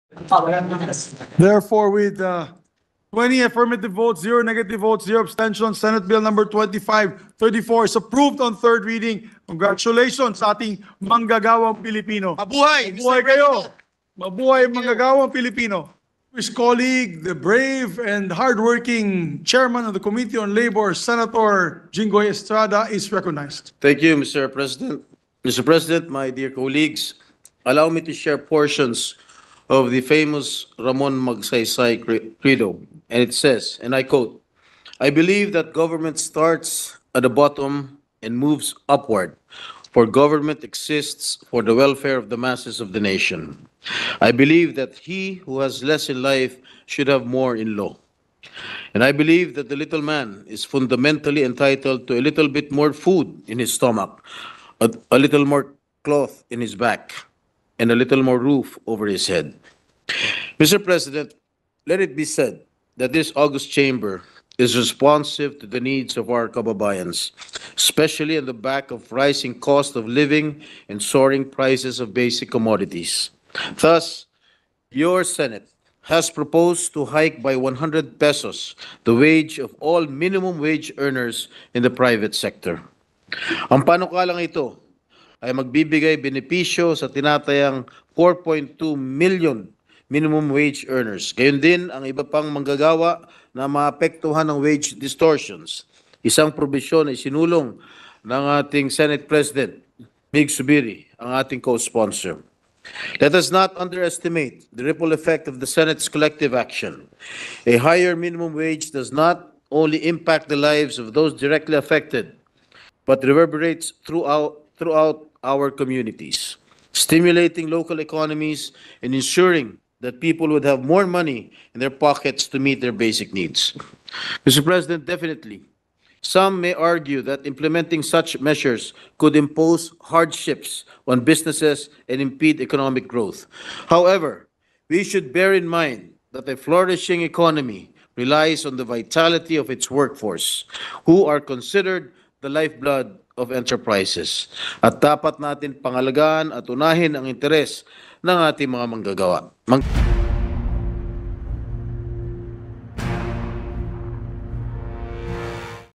₱100 taas sahod sa minimum wage earners pasado na sa Senado
Lumusot na sa ikatlo at pinal na pagbasa sa Senado ang Senate Bill 2534 o ang ₱100 legislated wage hike para sa mga manggagawa sa pribadong sektor.

Sa botong 20 senador ang pumabor, walang kumontra at wala ring nag-abstain, inaprubahan na ng Senado ang panukala para magbigay ng dagdag na sahod sa may 4.2 milyong minimum wage earners.